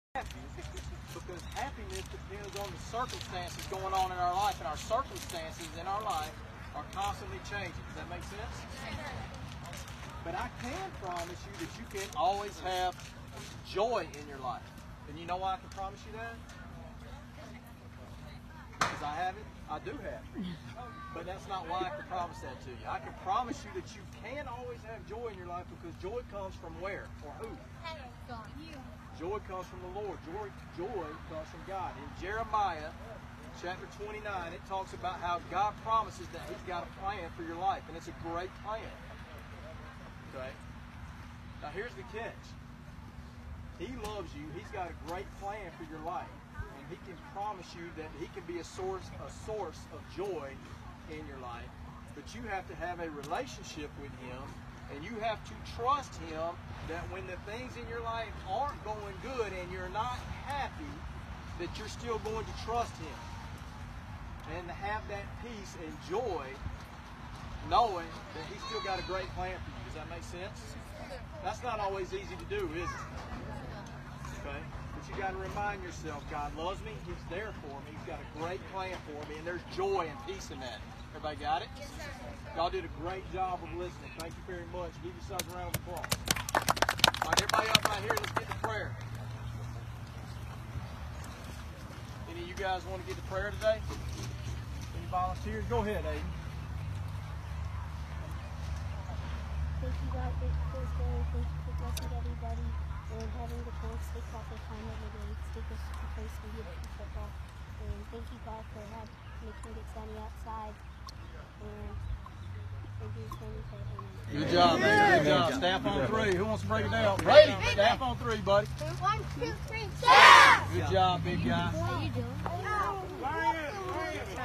STaF Train Up Talks: Finding Joy in God's Plan
STaF Athletics' Executive Director Chris Stafford teaches Jeremiah Chapter 29 to young Christian Athletes. Chris talks to the athletes about building a relationship with Christ and trusting God's plan. By trusting the Lord's plan for you, there's never a question of where your joy comes from!